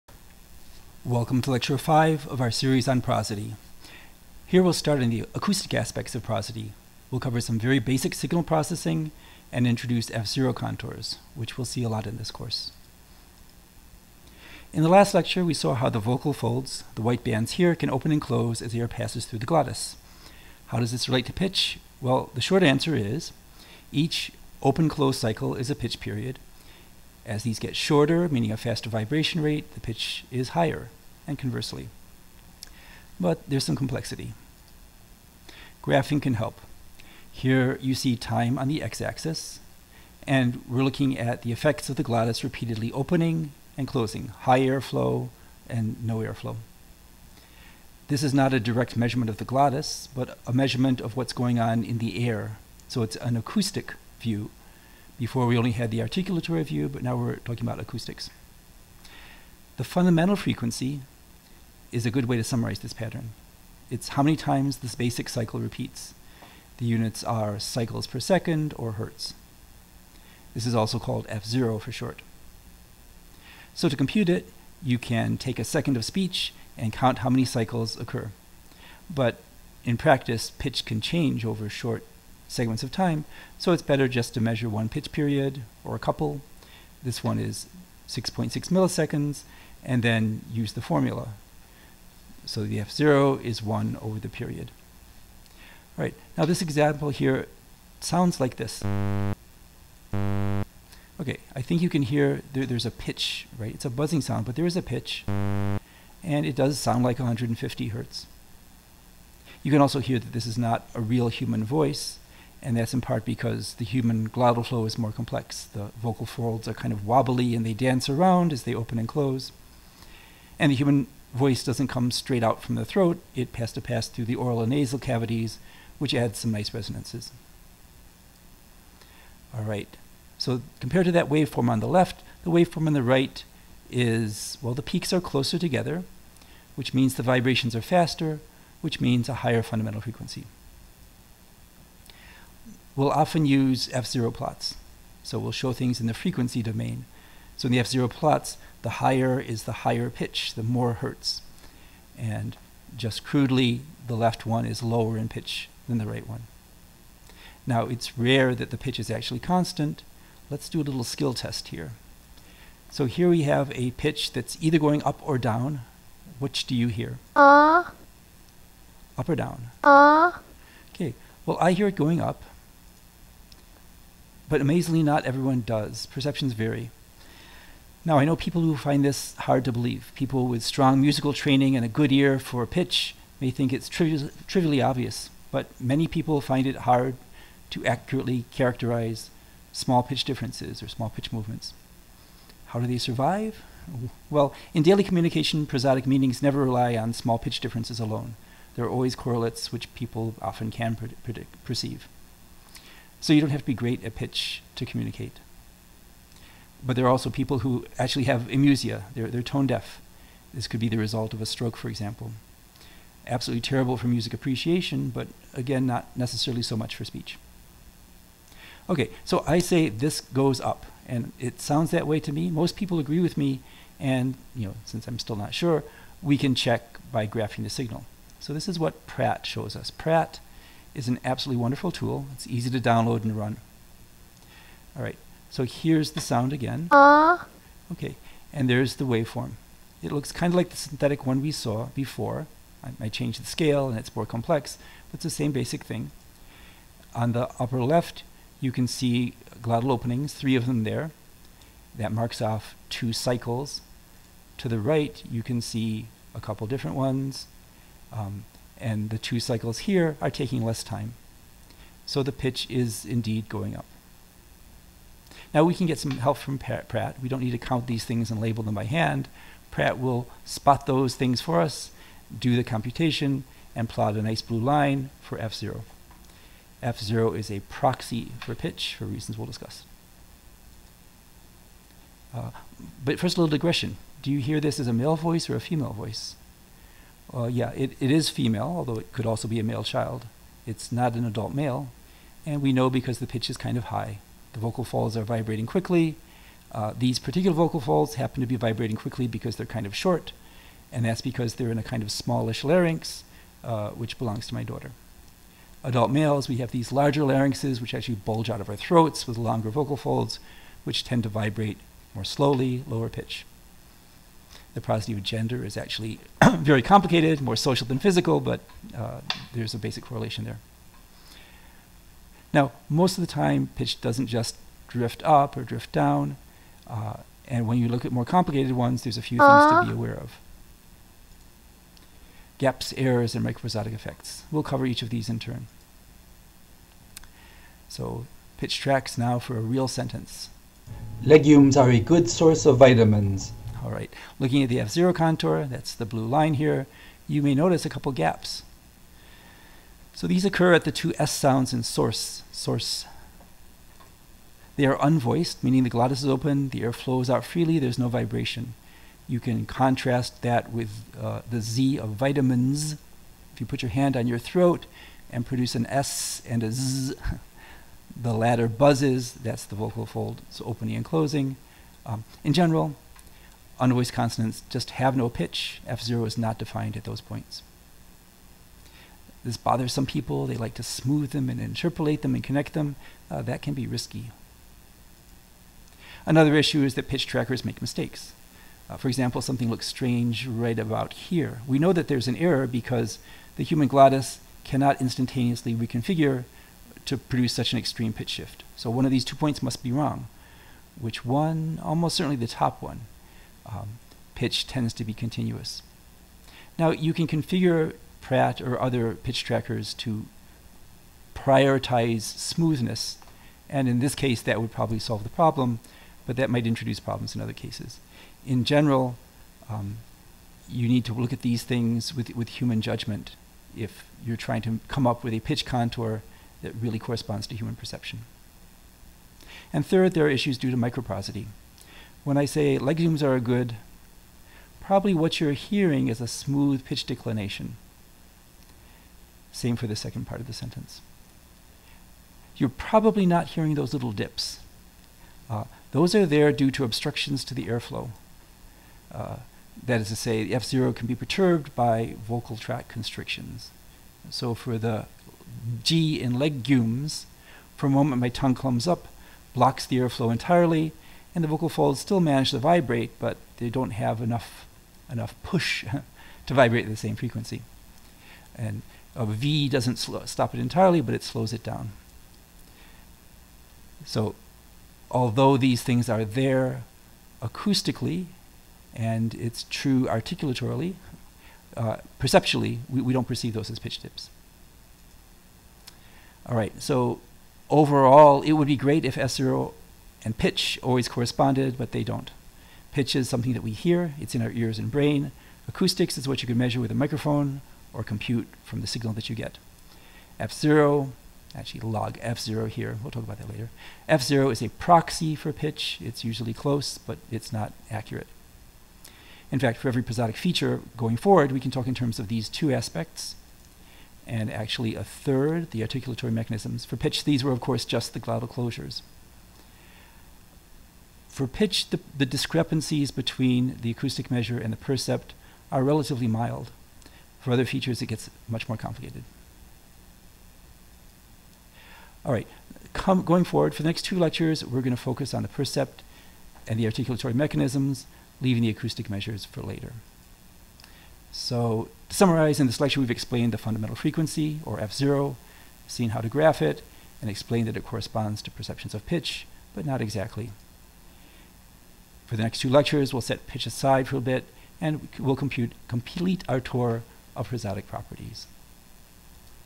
Prosody Tutorial: Lecture 5: Pitch and F0
This is Video 5 of our series on prosody. We explain how the F0, computed directly from the waveform, corresponds well to both the glottal cycle and to human perceptions of pitch.  Nevertheless, microprosodic effects make the interpretation of F0 contours tricky.  Ultimately, pitch, like every prosodic property, has articulatory, perceptual, and acoustic aspects, which don't perfectly coincide.

00:00   Waveforms and Pitch Periods
00:35   The Fundamental Frequency (F0)
02:40   Pitch Perception can be Hard
03:48   F0 Contours and How to Read Them
05:22   Divergences between Pitch Percepts and Computed F0
07:09   Microprosody
08:05   The Articulatory-Perceptual-Acoustic Triumvirate